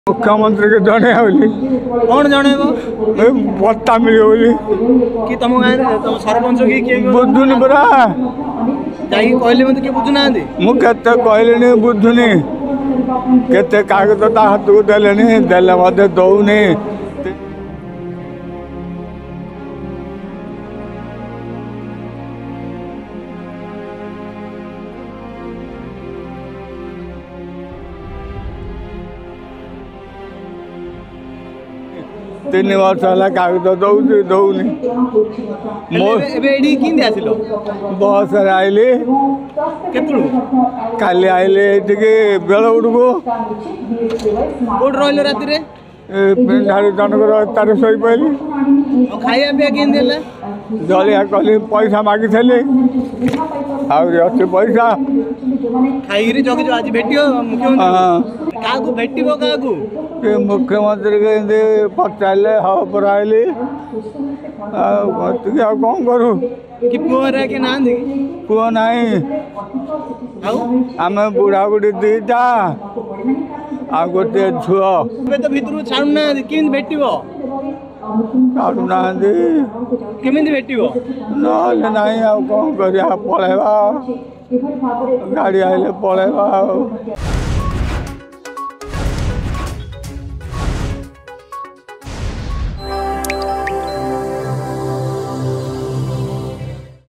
ମୁଖ୍ୟମନ୍ତ୍ରୀଙ୍କୁ ଦୁଃଖ ଜଣାଇବାକୁ ଆସିଥିଲେ ଅସହାୟ ବୃଦ୍ଧ
ନିଜ ନିର୍ଵାଚନ ମଣ୍ଡଳୀ ହିଂଜିଳିକାଟୁରେ ମୁଖ୍ୟମନ୍ତ୍ରୀଙ୍କୁ ଦୁଃଖ ଜଣାଇବାକୁ ଆସିଥିଲେ ଜଣେ ଅସହାୟ ବୃଦ୍ଧ।